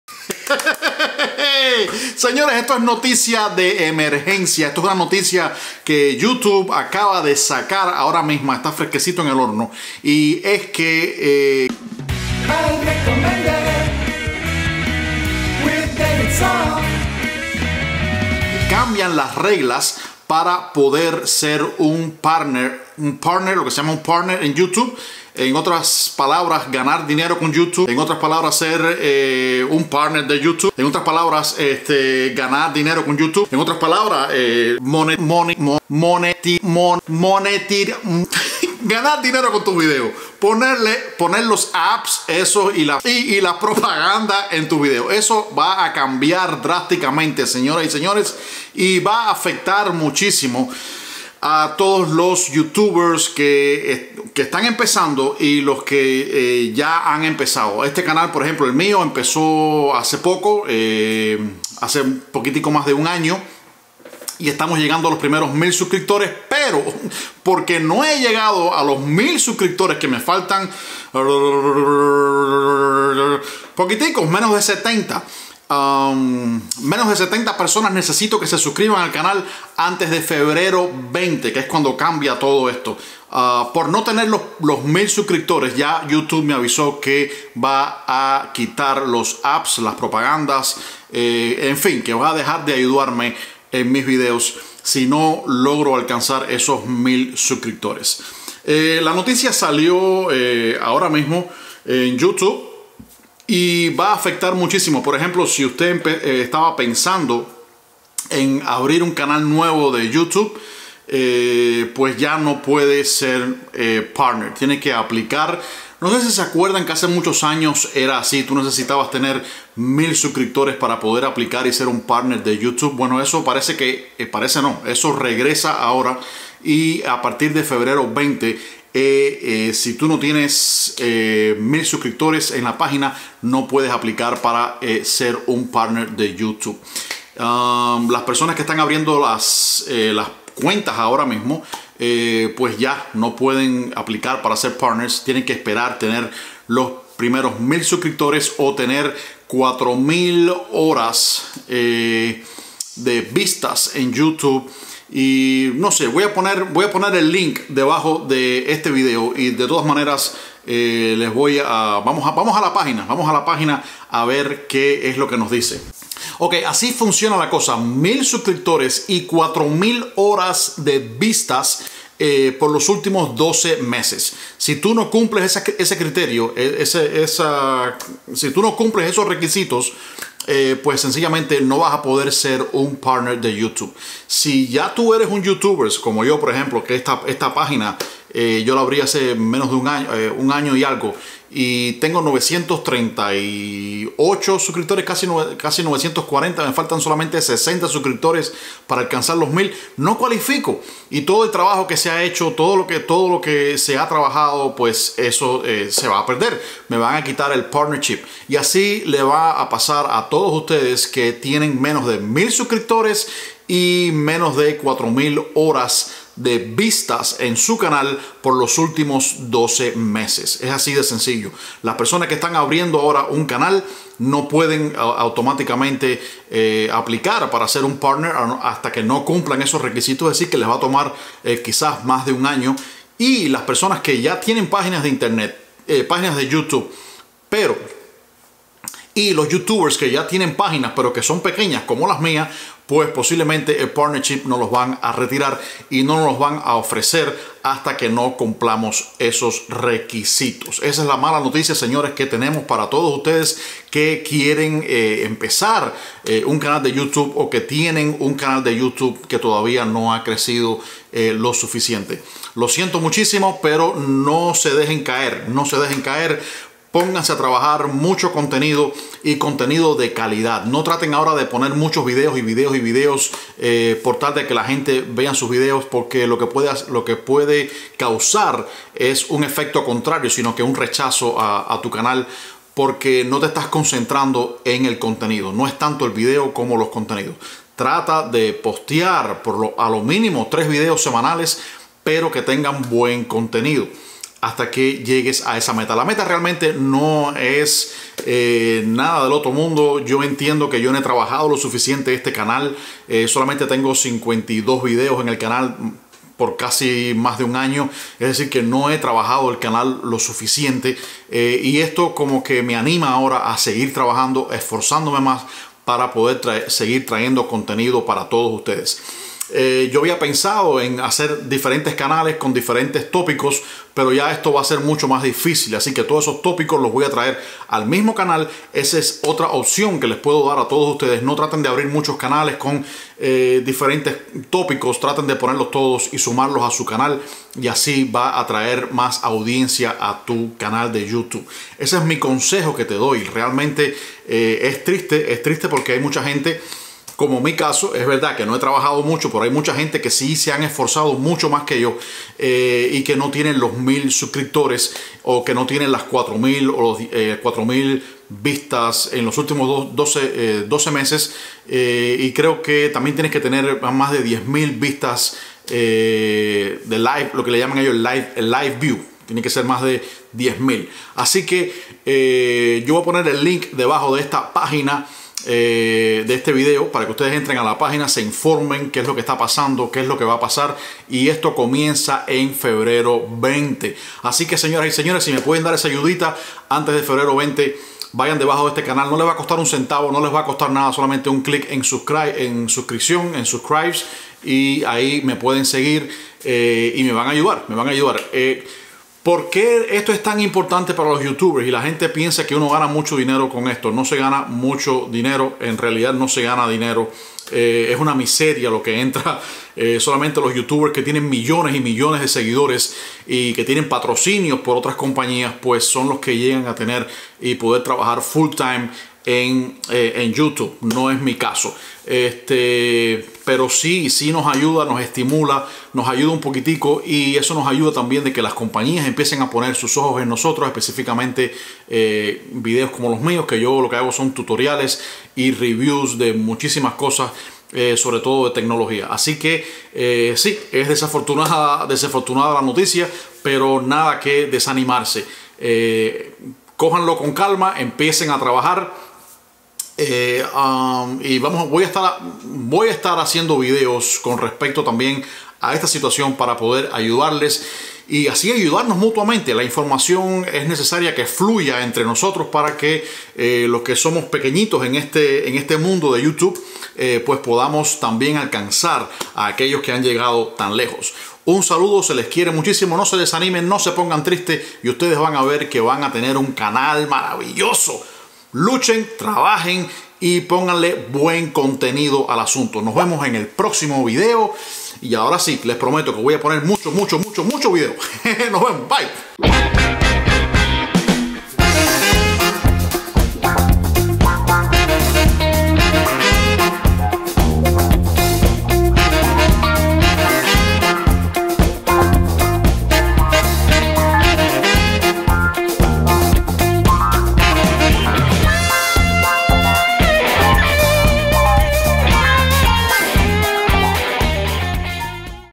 2018 Se Termina Youtube
Youtube ha anunciado un cambio en sus pólizas para los creadores de contenido y sera efectivo a partir de Febrero 20. Conectate para enterarte.